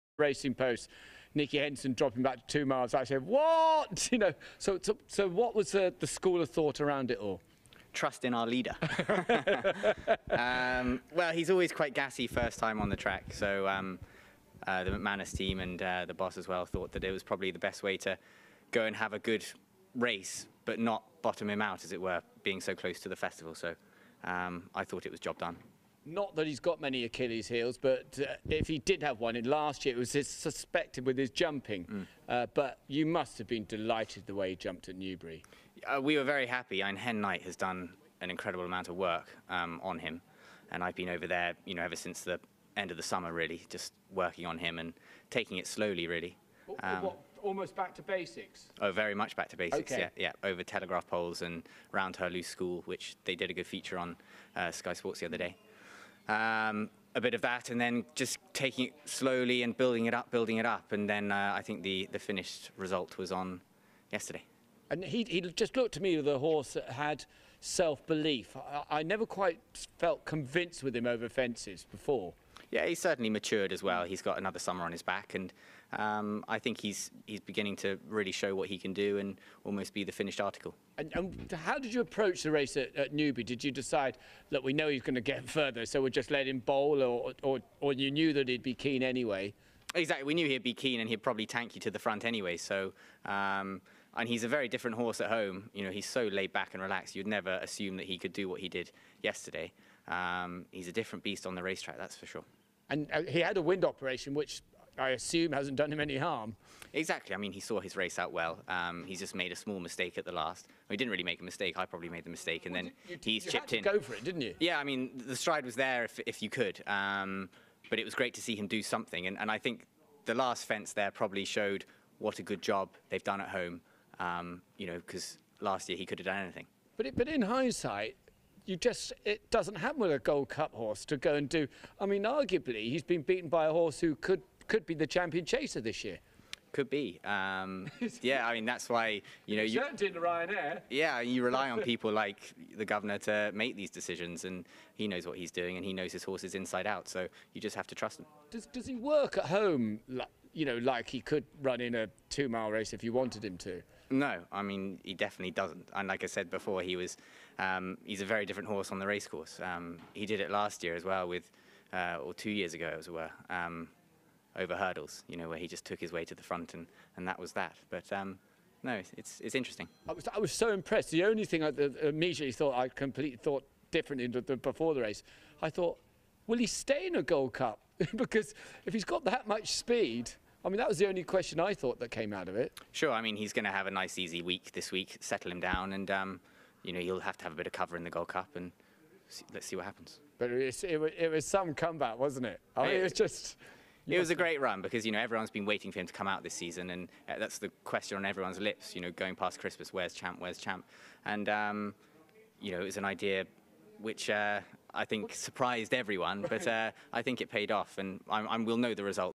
Roll on the Gold Cup! Nico de Boinville chats Champ's prep for Cheltenham
Reaction to Champ's comeback from the man on board! Nico de Boinville speaks to Luke Harvey on Sky Sports Racing about the RSA hero, and how the "Guvnor" Nicky Henderson is prepping him for the Gold Cup in three weeks time...